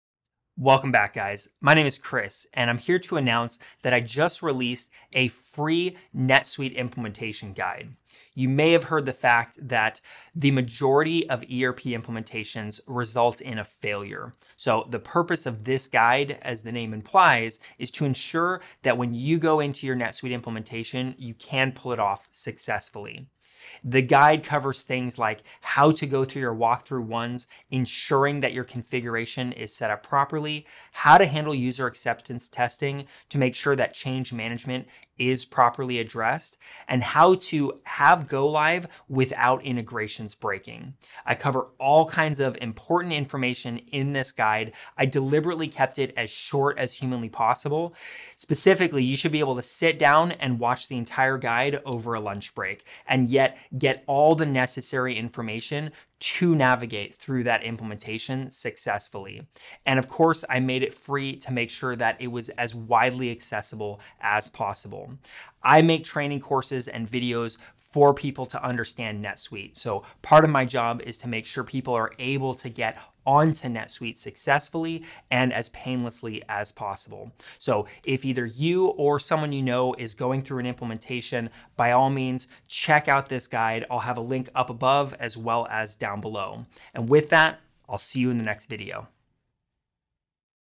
NetSuite Implementation Guide
In this video, I announce the release of my new NetSuite Implementation Guide. This free guide is just over 30 minutes long and covers all the basic information any client should know for a successful Go-Live.